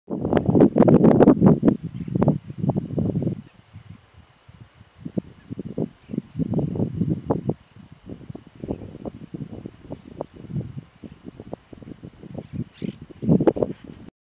Dama Pintada
Mariposas Dama Pintada (Vanessa braziliensis) filmadas durante una recorrida por la zona rural de Funes.